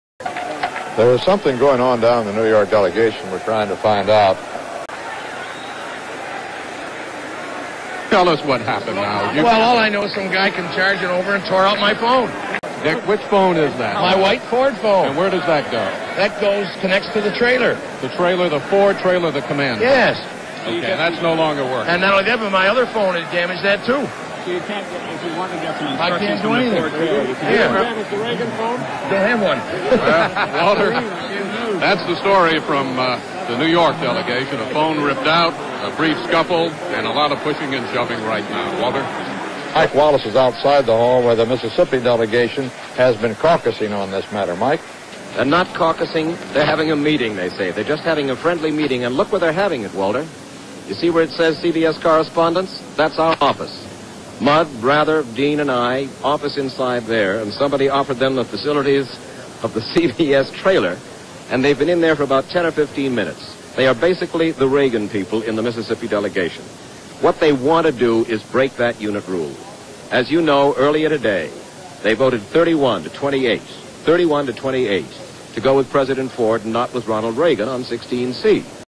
1976 GOP Convention
Snippet from CBS coverage of 1976 GOP Convention, with Walter Cronkite, Morton Dean and Mike Wallace.